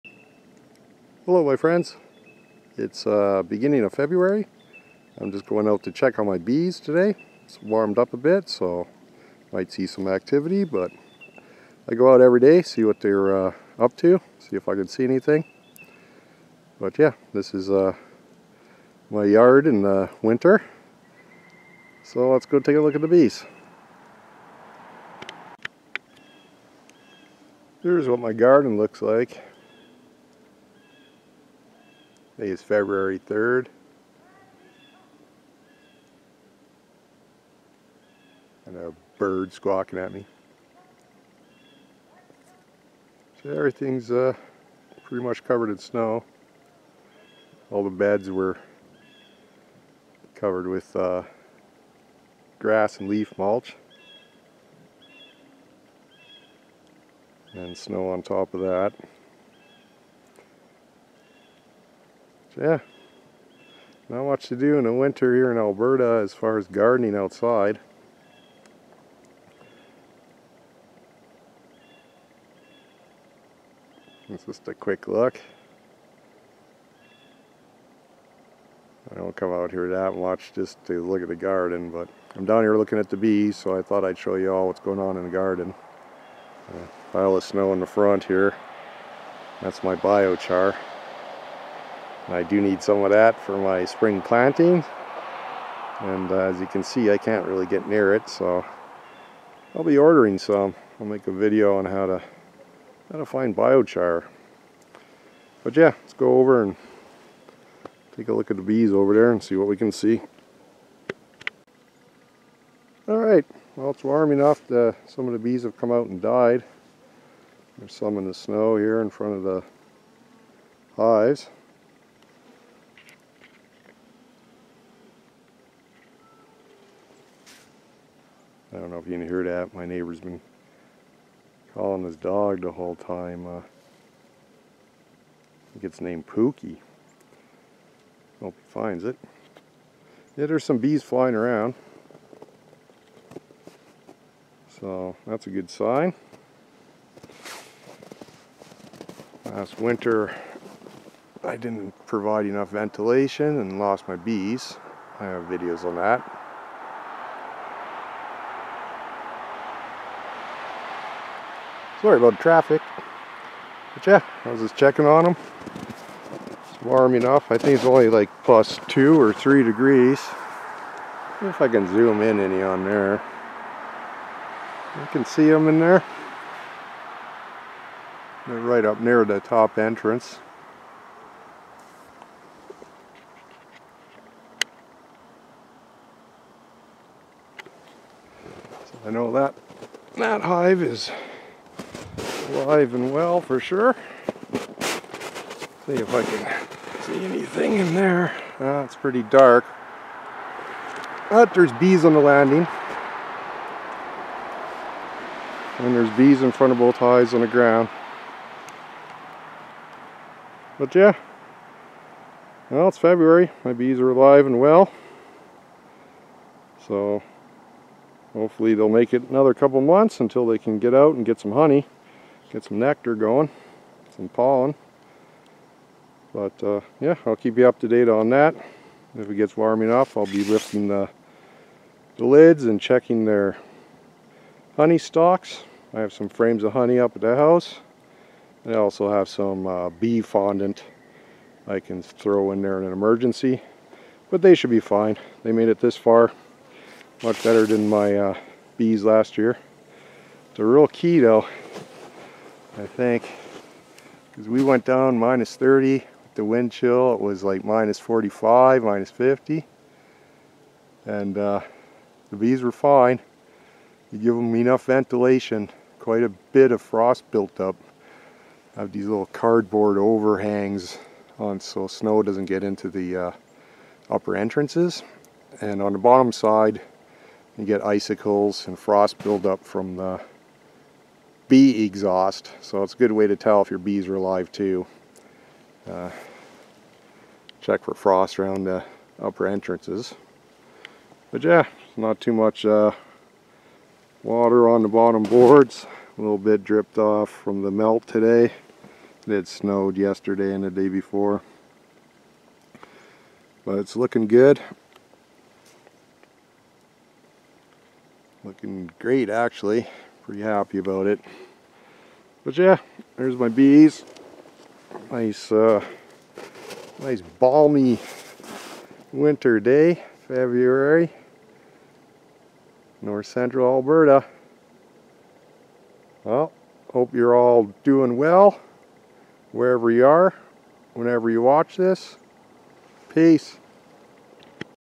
Bees survive Alberta Winter. 2023
So far so good for my bees to start February.

Wintering bees in Alberta.
Ventilation and insulation are key to making it easy for the bees.